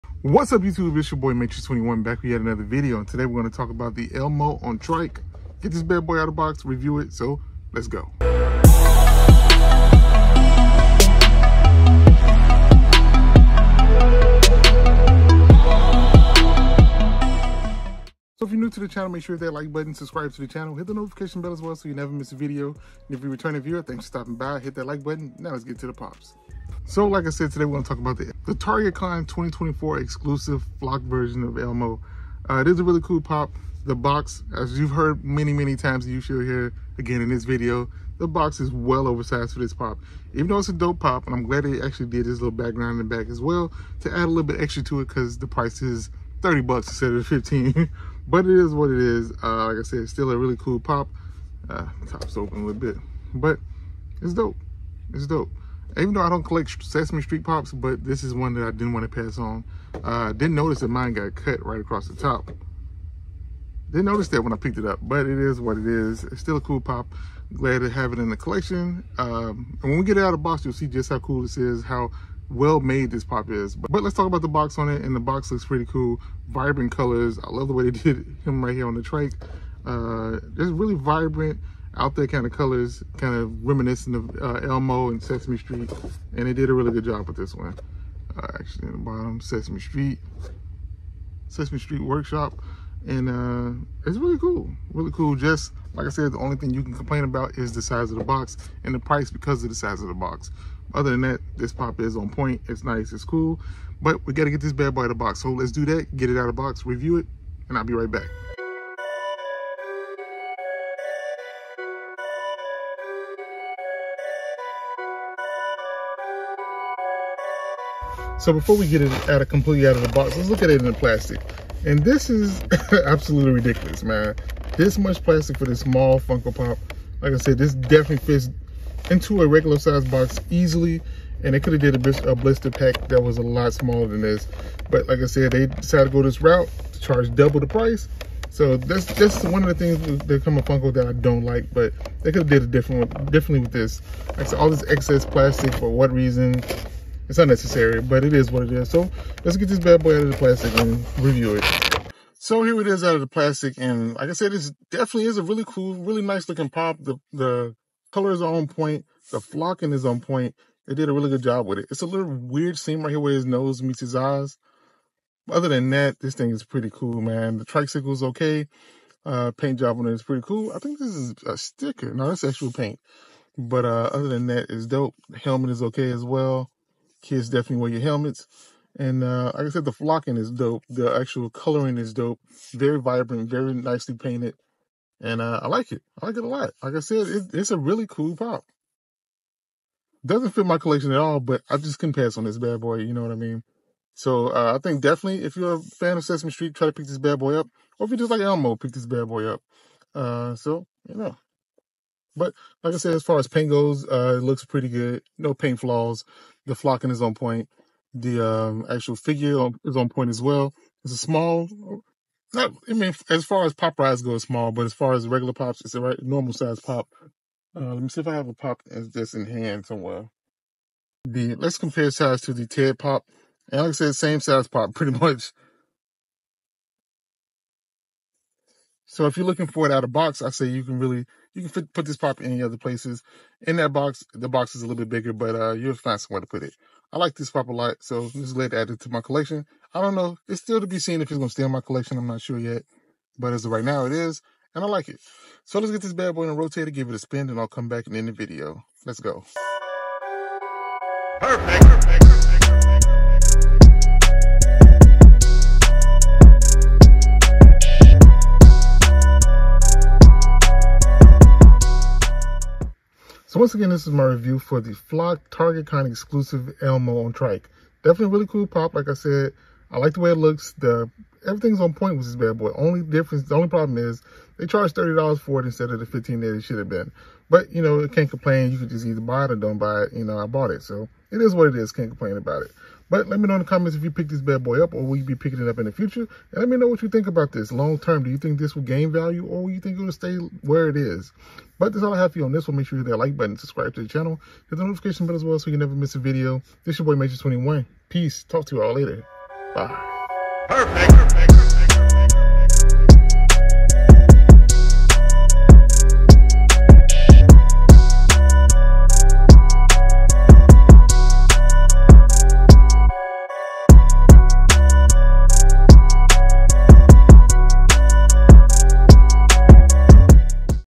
Elmo on Trike Funko Pop Out of Box Review
Let's take a look at the new Targetcon Exclusive Flocked Elmo on Trike Funko Pop

Fun of the Hunt T-Shirt